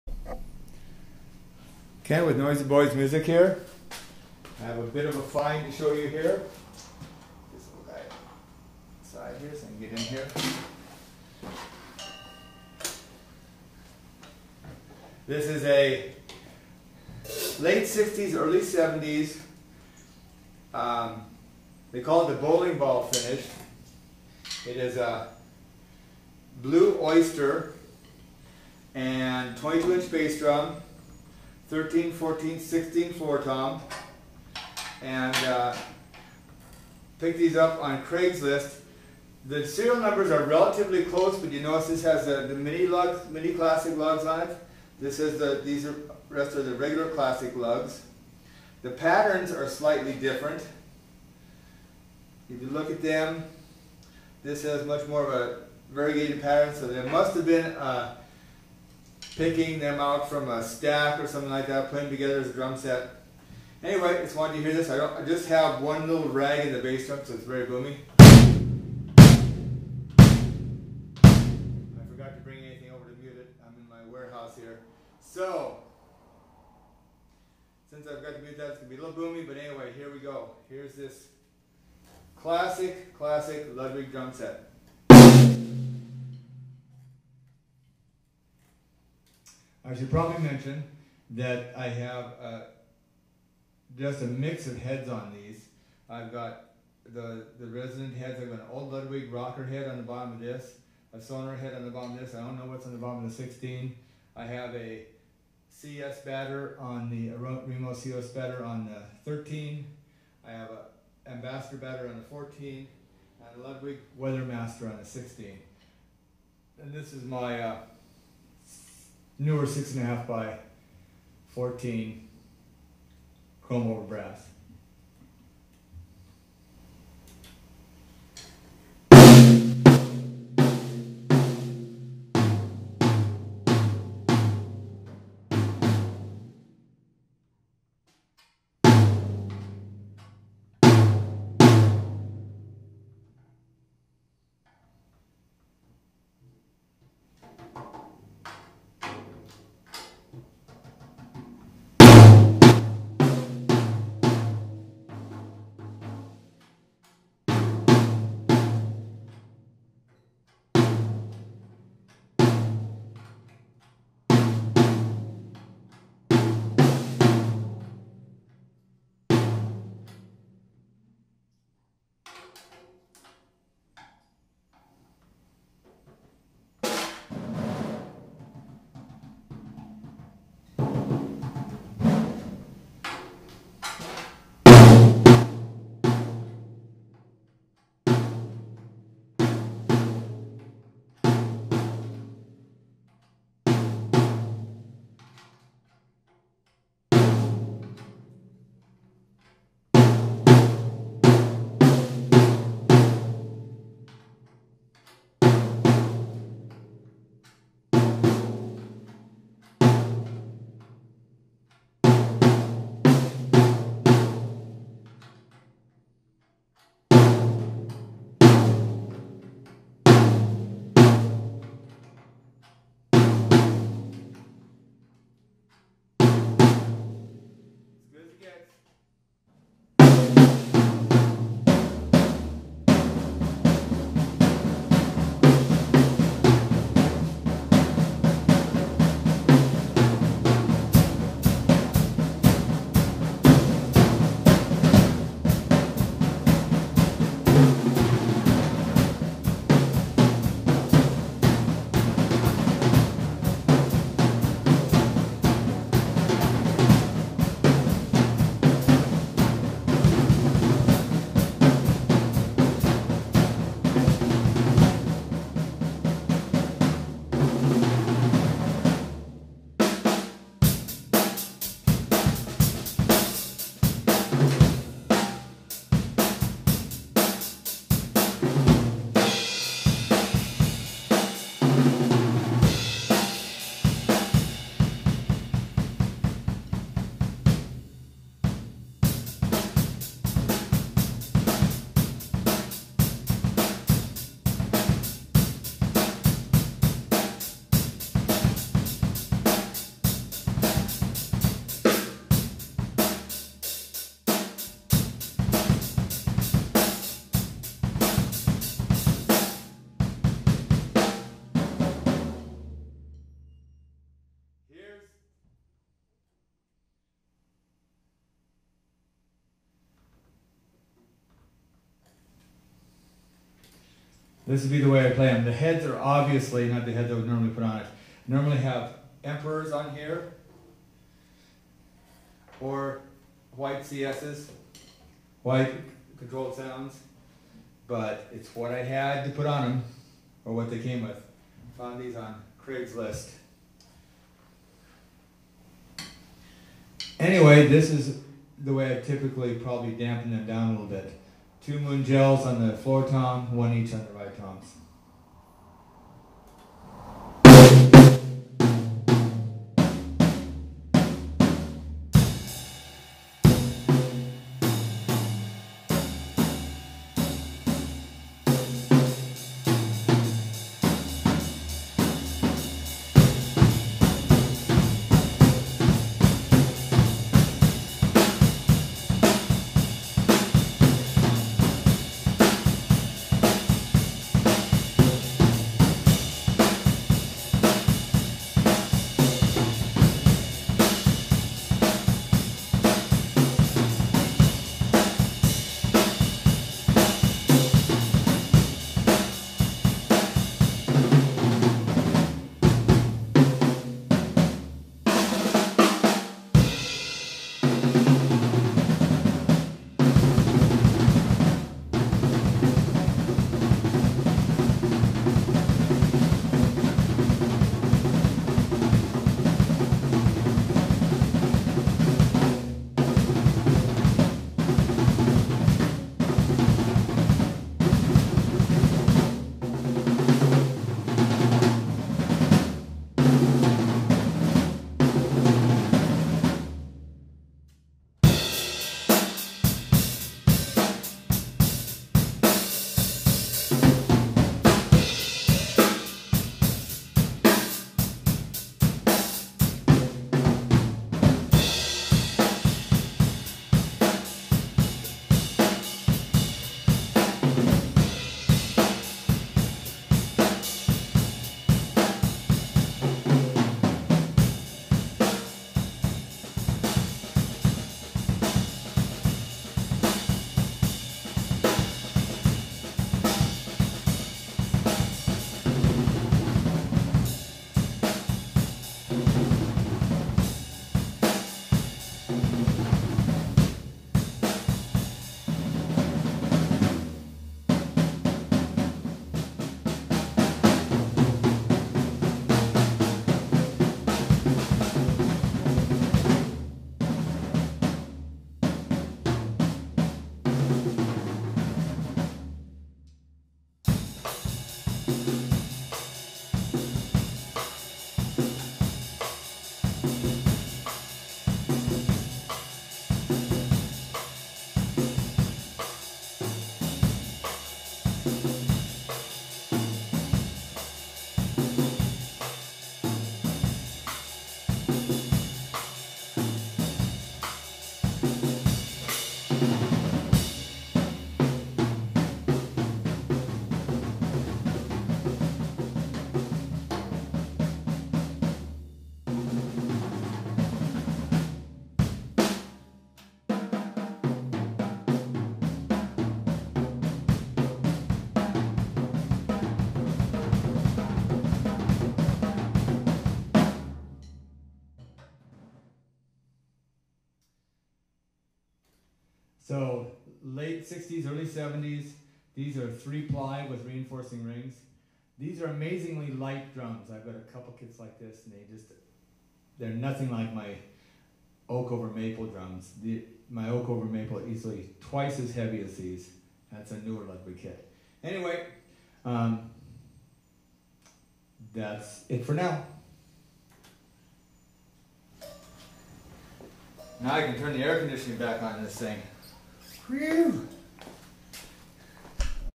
Vintage late 60s early 70s Bowling ball ludwig drum kit (Blue Oyster). 13/14/16/22 3 ply
I decided to put together some short videos (since my drum expertise doesn't justify longer ones) share philosophy of drum setup and playing. Enjoy!  If you have any questions or comments fire away; if there's enough I'll make a video to address them or message you back.